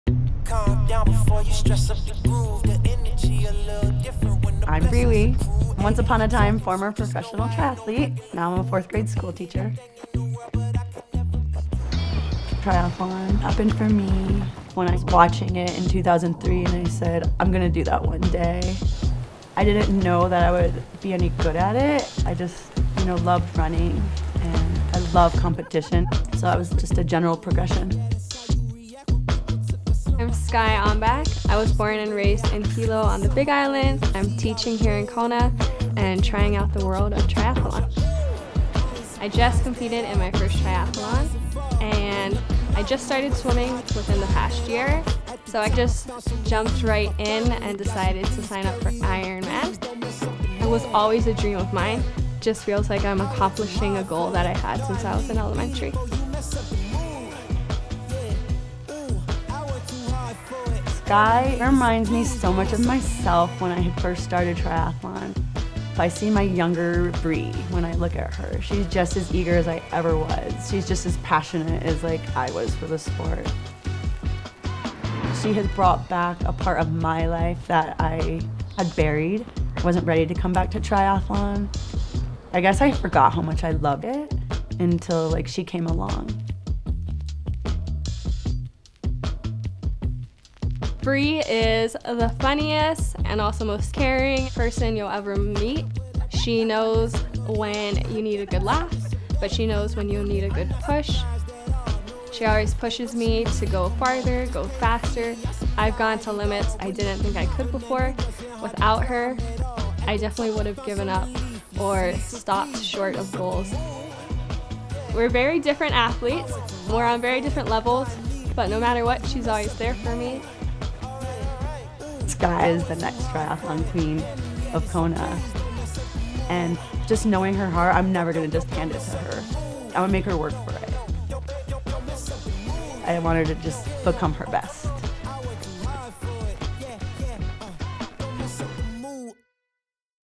Humans of HOKA: Bree & Skye
To many people, triathlons and ultra marathons can be intimidating, but the right support system proves anything is possible. Bree Wee and Skye Ombac are working together to accomplish things neither thought possible.